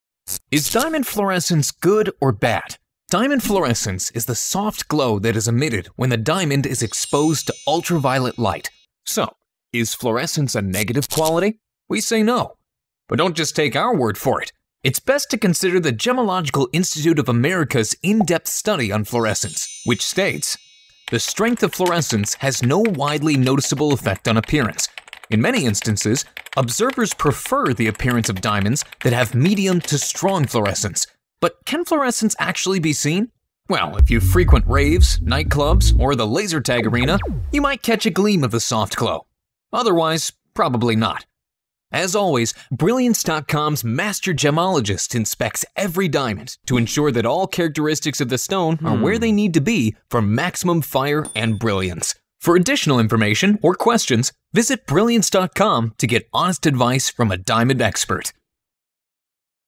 Is Diamond Fluorescence Good or Bad?
Fluorescence is the tendency of a diamond to emit a (soft) glow when exposed to ultraviolet light (UV light). The fluorescence effect is present in over 30% of diamonds and is an important consideration when buying a loose diamond.